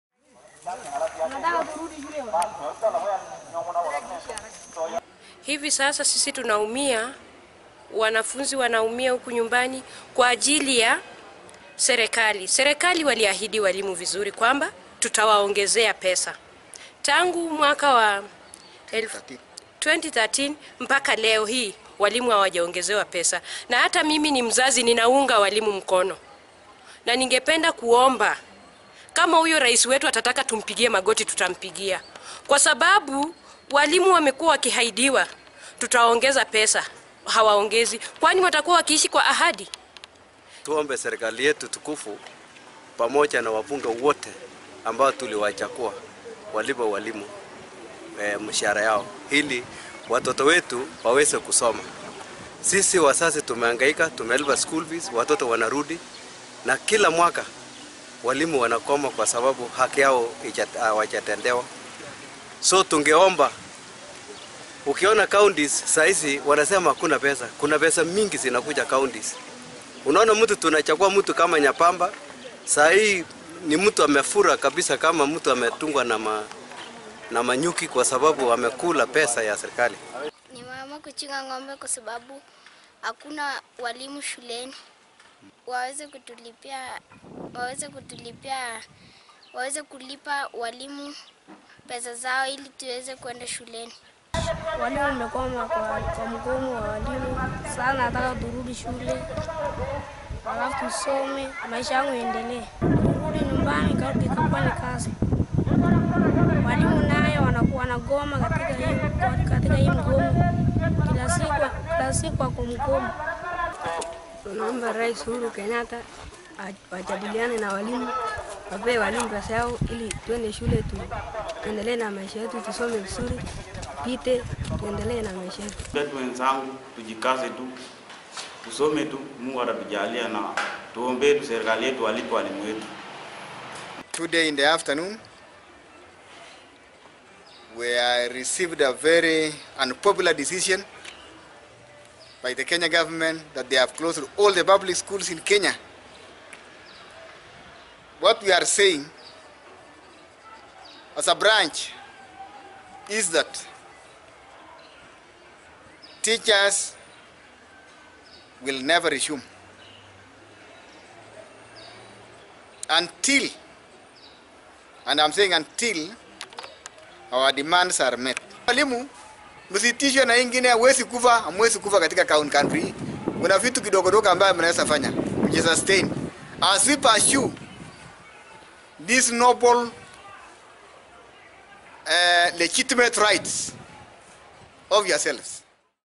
Learners resort to grazing cattle as teachers asked to engage in other businesses.
Learners in West Pokot have resorted to looking after cattle following the ongoing teachers strike in the country. This comes after the government closed all schools in the country .
In school visited in the county, most schools in West Pokot county remain closed as classrooms remain deserted and animals were  seen grazing within the school compound. Speaking to the press ,Knut West Pokot Executive Secretary Martin Sembelo, faulted the government for closing schools citing that it was a wrong decision. Sembelo called on teachers in the county to engage in other businesses. He further added that teachers will not resume until their demands are met.